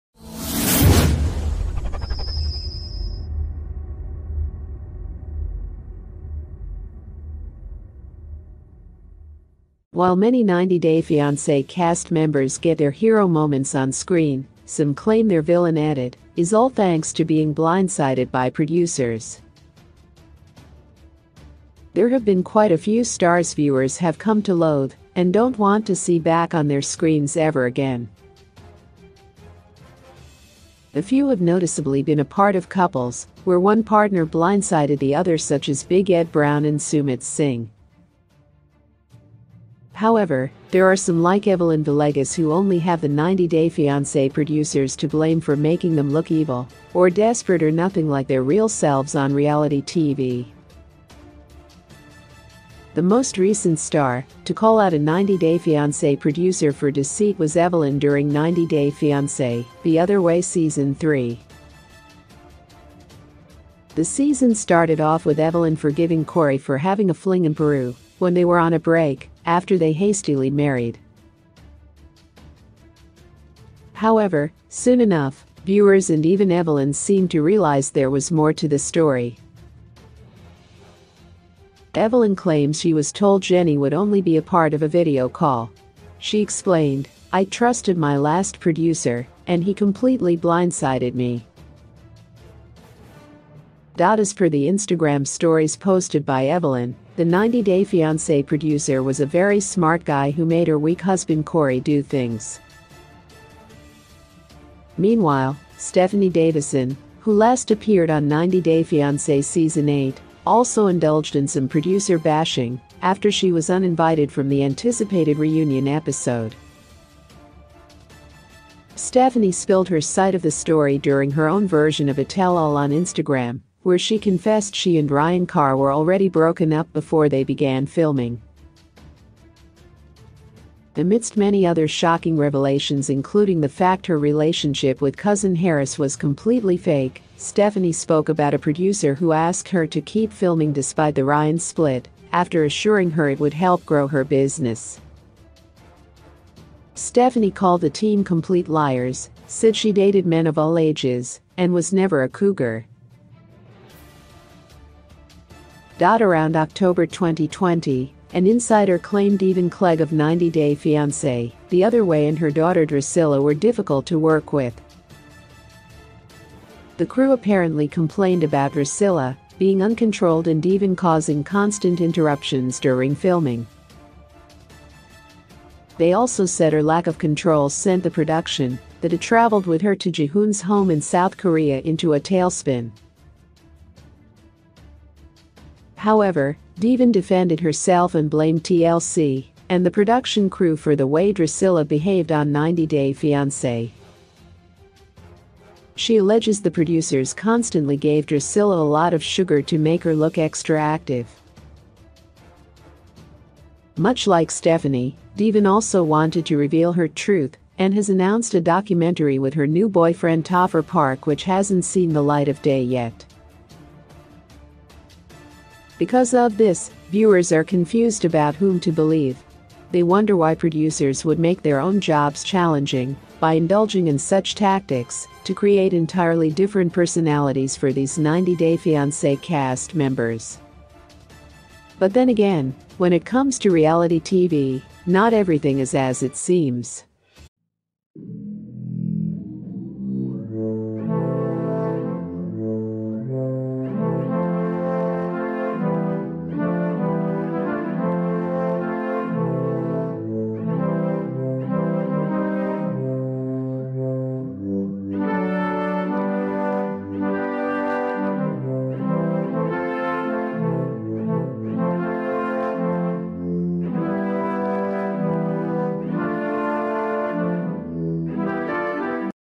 90 Day Fiancé: Cast Members Who Were Blindsided By Producers
90 Day Fiancé Cast Members Who Were Blindsided By Producers.
90 Day Fiancé:
90 Day Fiance offers a unique look into the world of international dating and matrimony.